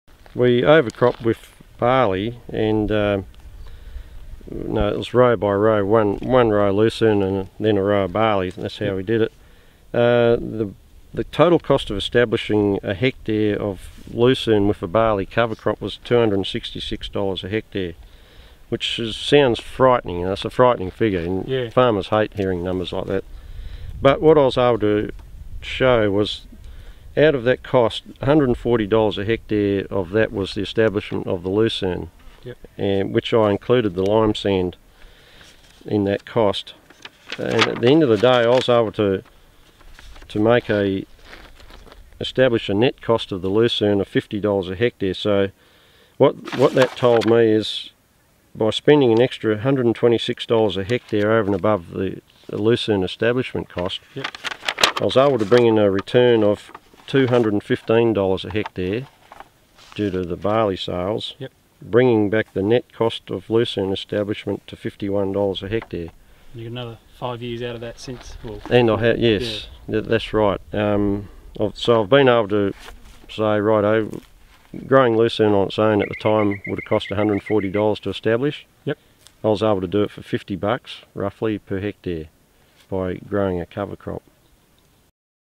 Offsetting the costs of lucerne establishment
Gavin Morgan on how he used a barley cover crop to offset the establishment costs of lucerne